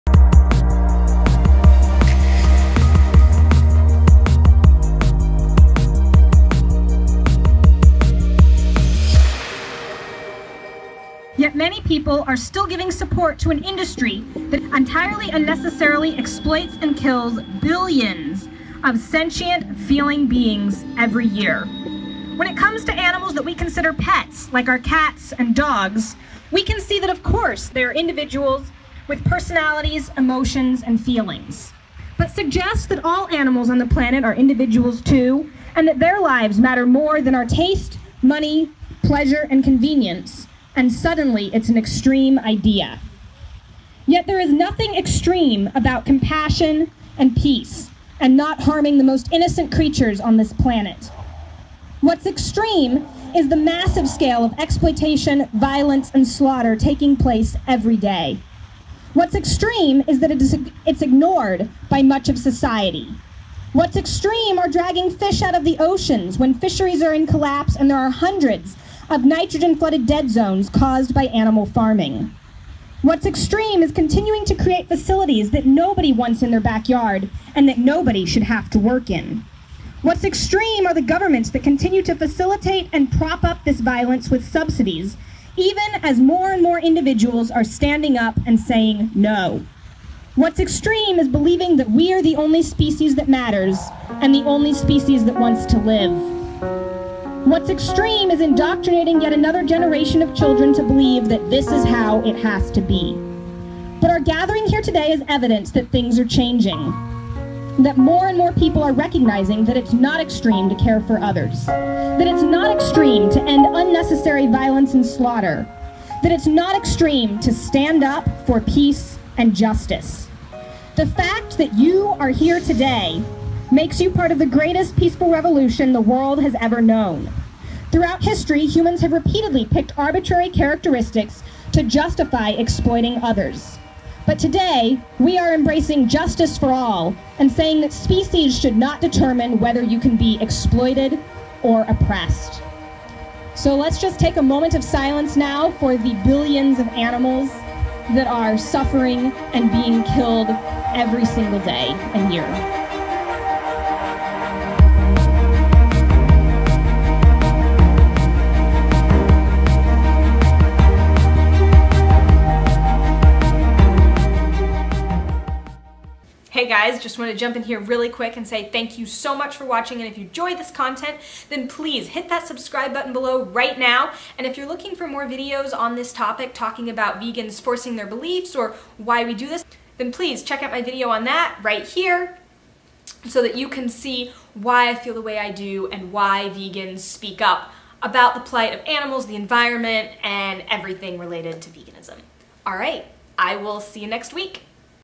Is Veganism Too Extreme?
Most people already agree that it's wrong to hurt animals unnecessarily, yet most people still support violence towards animals on a daily basis. So what is really more extreme...being vegan, or killing innocent animals for pleasure?

This is the speech I gave at Kansas City's first National Animal Rights Day event!